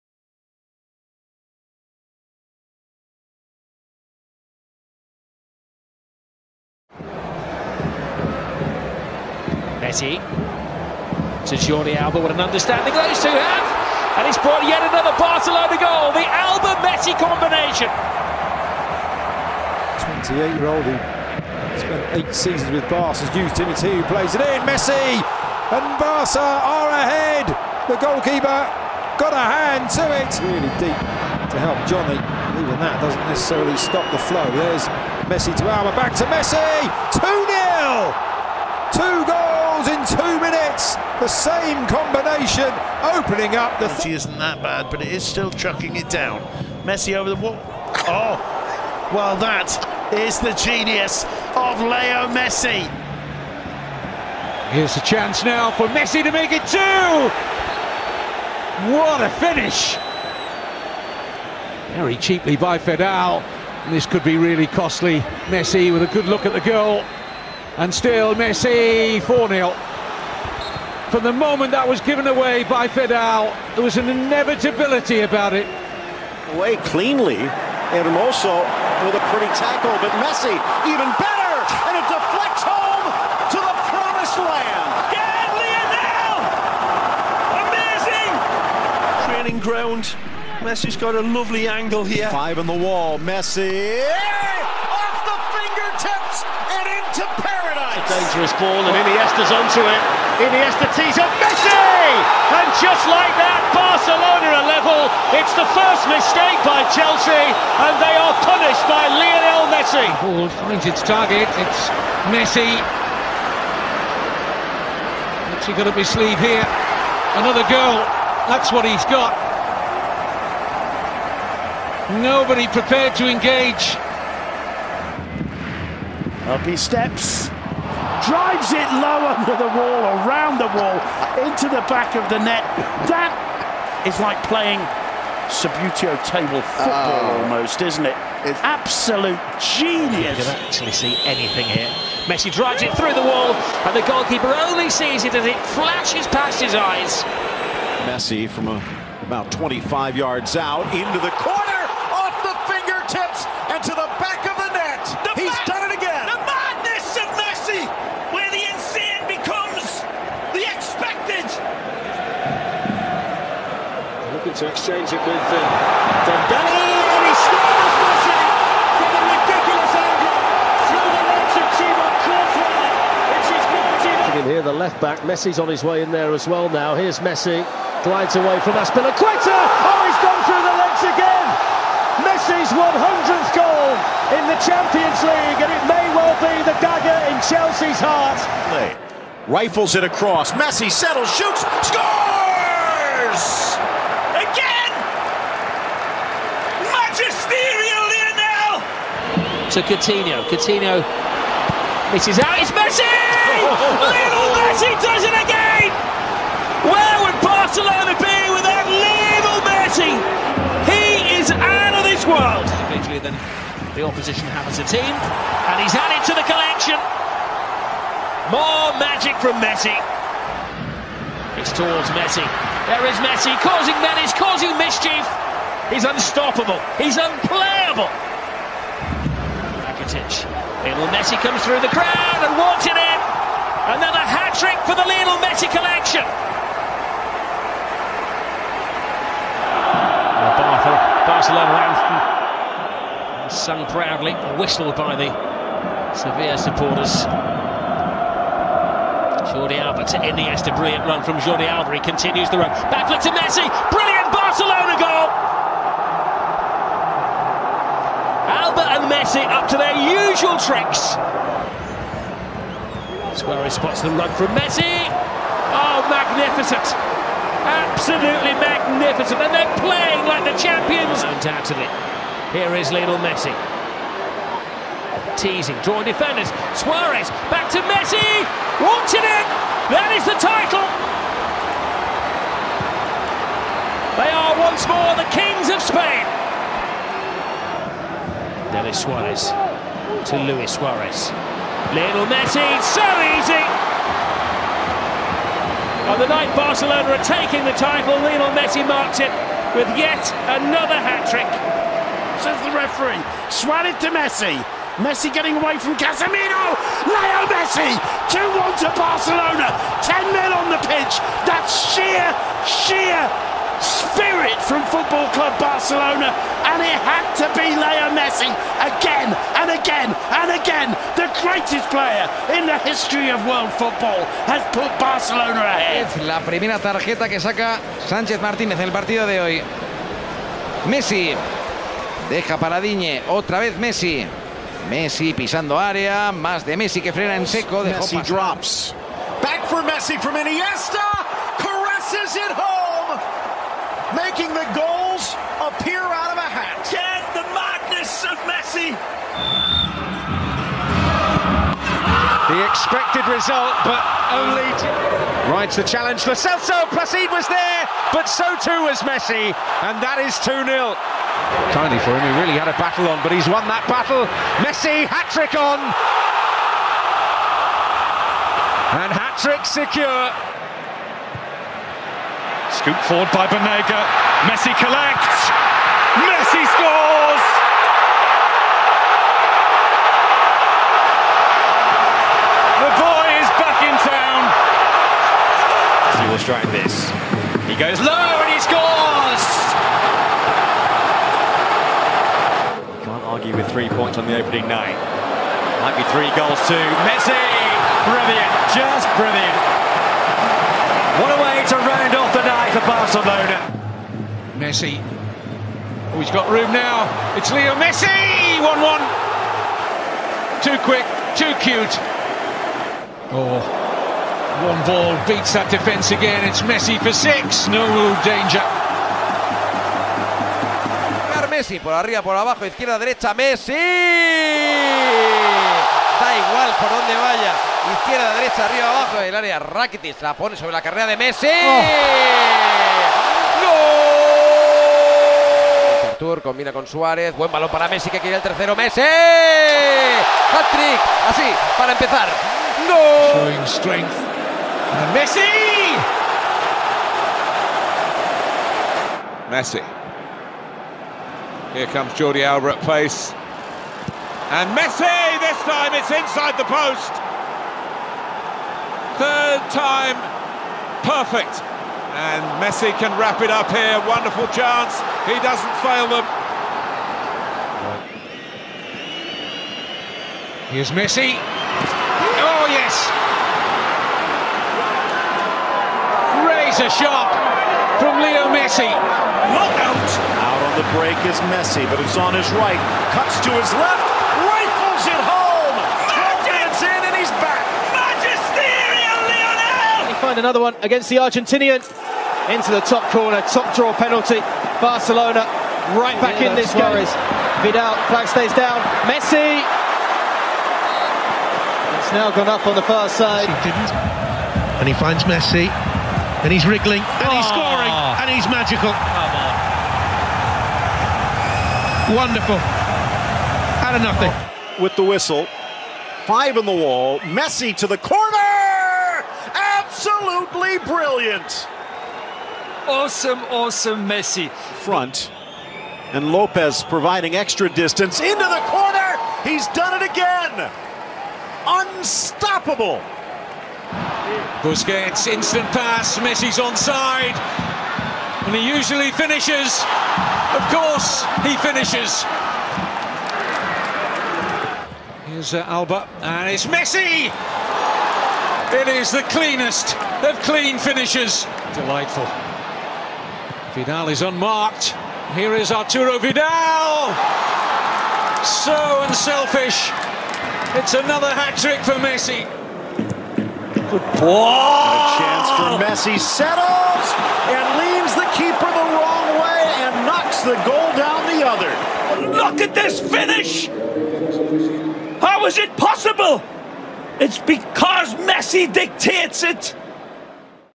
Lionel Messi ● All 51 Goals in 2018 ● With Commentaries
All of Messi's goals in 2018 with commentaries.